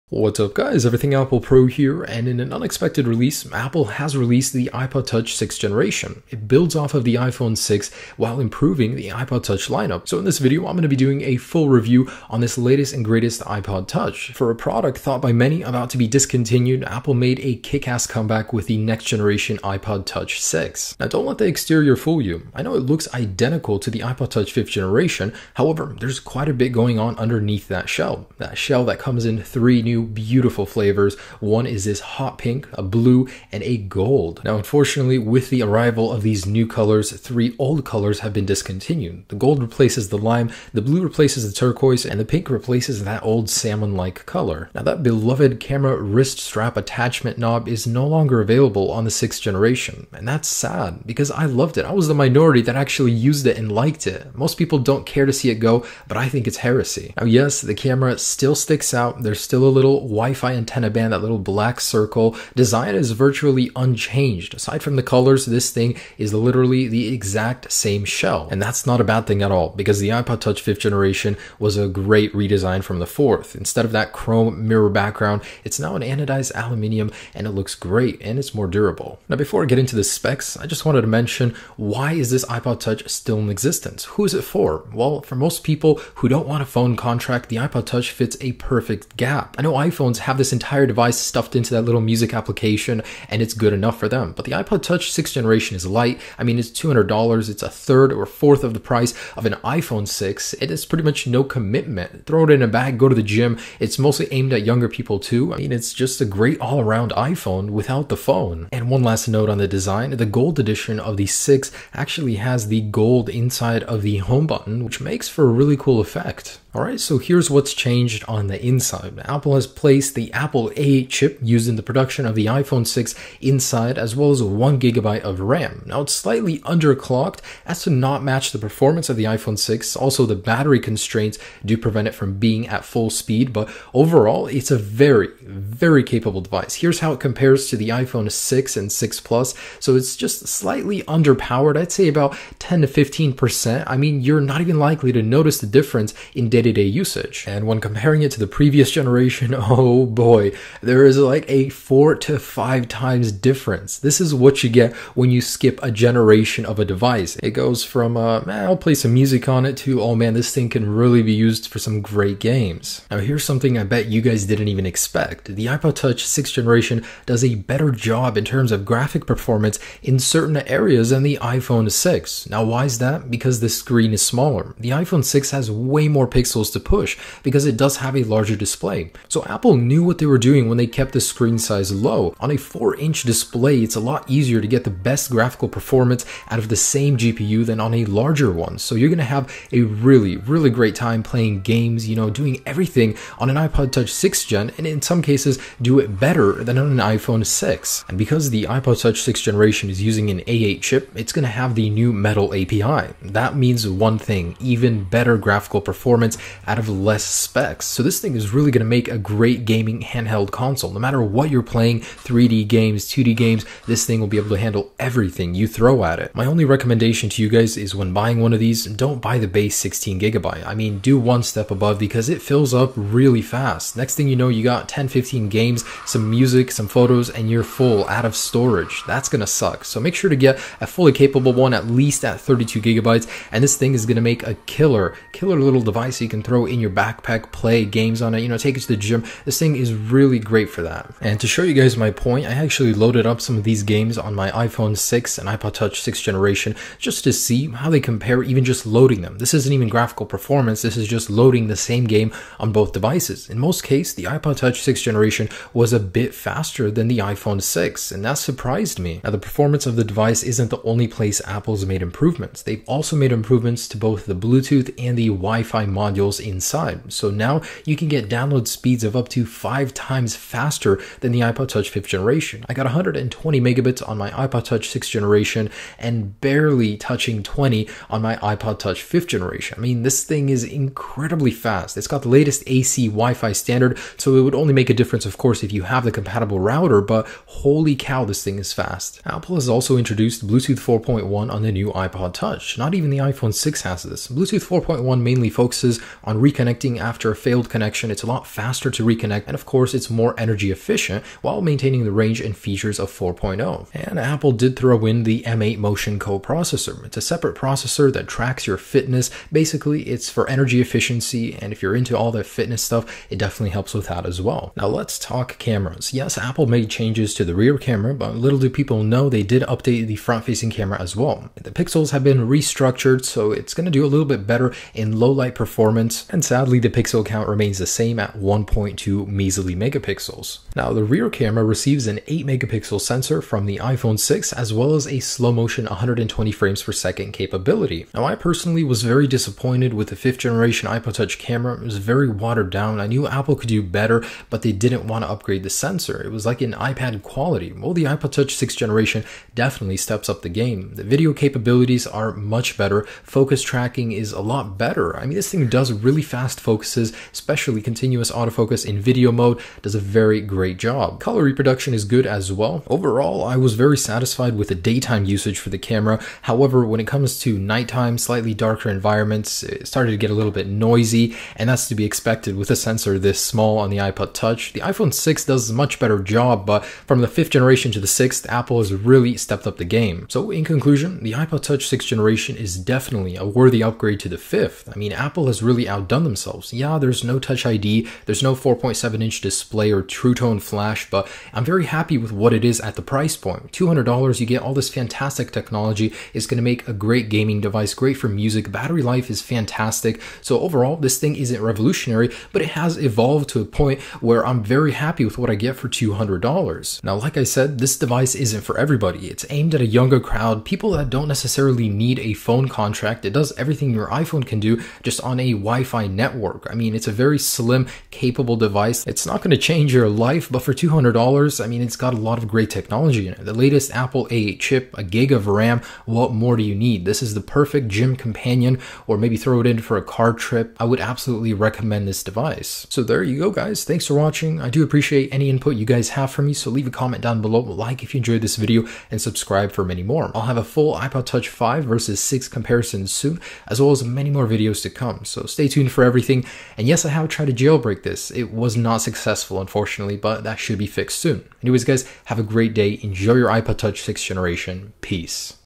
iPod Touch 6th Generation Review - 2015 iPod Touch 6G
All New Apple iPod Touch 6th Generation Review. Ultimate iPod Touch 6th Gen Detailed Review. 6G Camera, Speed Test, Puppy Test, Features & More.